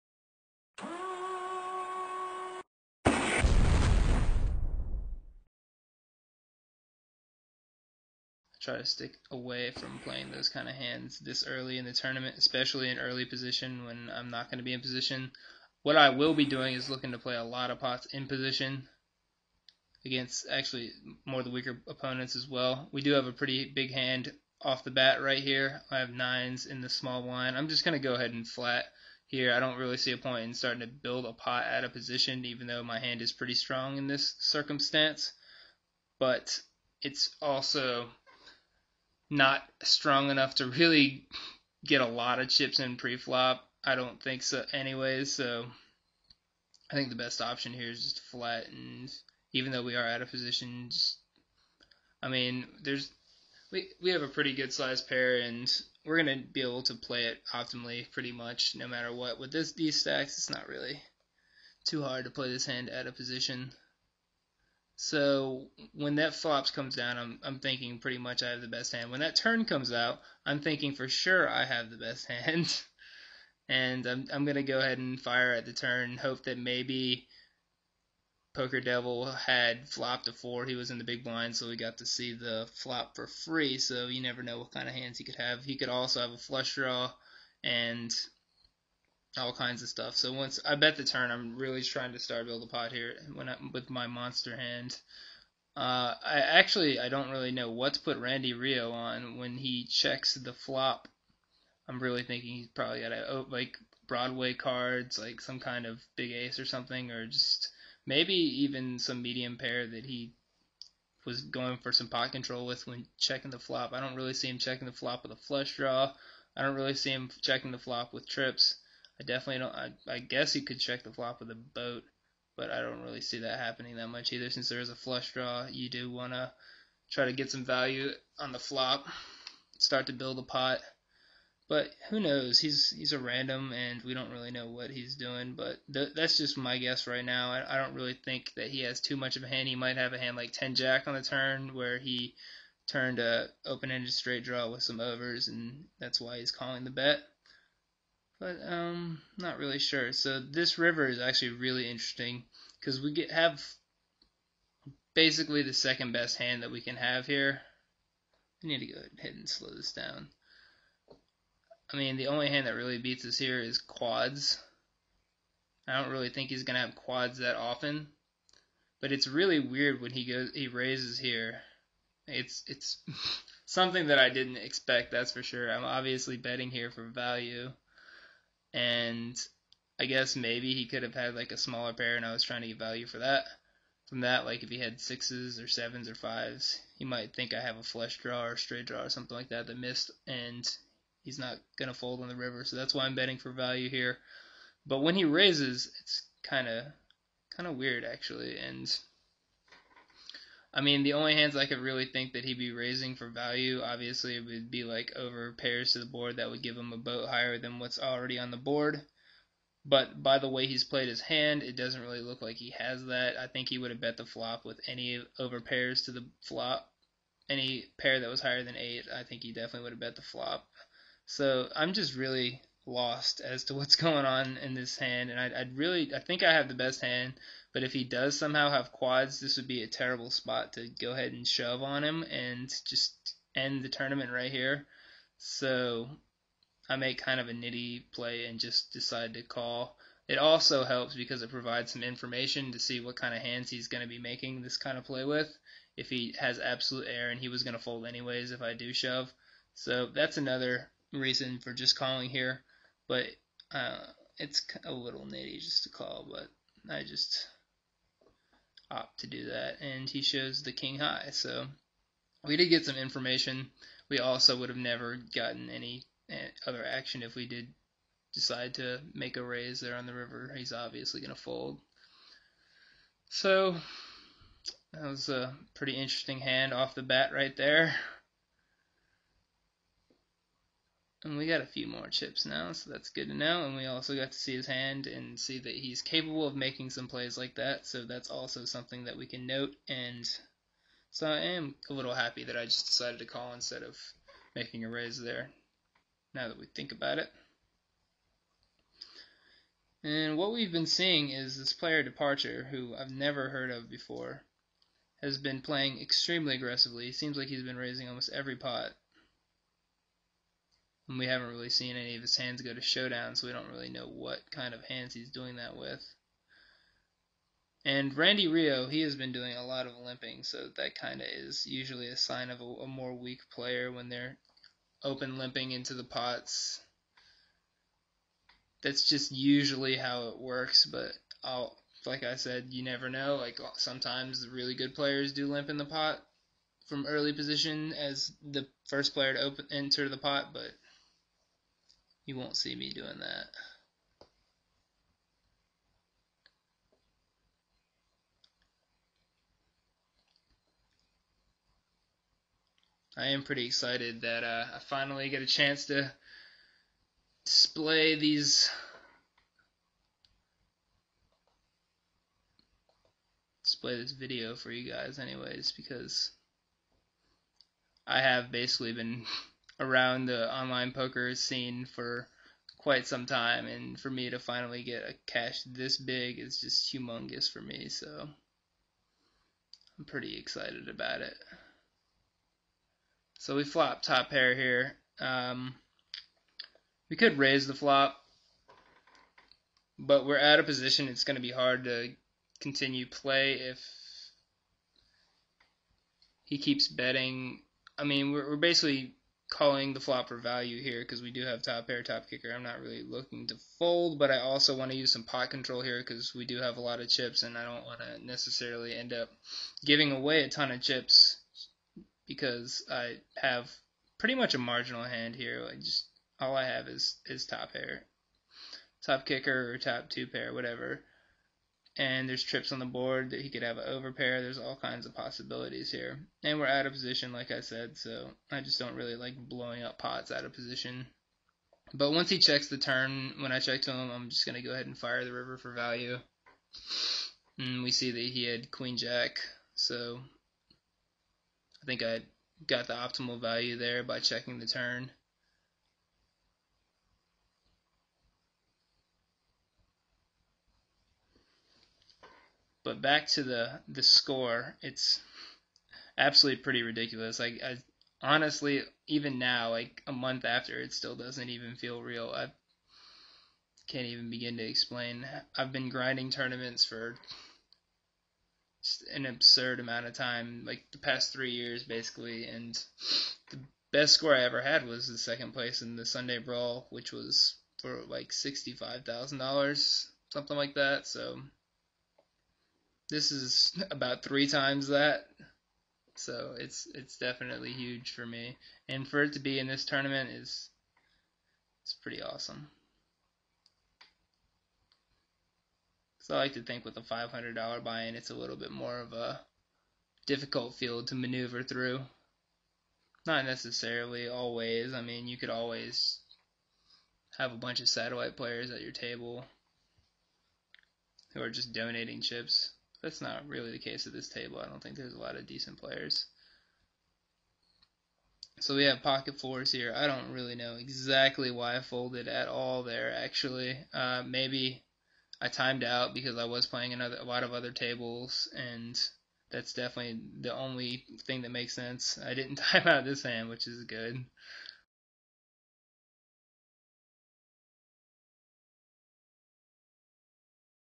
FTP Sunday Million Win Part 1 (Video 2)
In this series I will discuss every hand that I played in my path to a huge $180,000 win. Throughout the vidoes I will focus on concepts such as identifying weak opponents, obtaining maximum value, using creative lines, avoiding bleeding chips, adjusting to changing table dynamics, 3 and 4-betting, and selective aggression.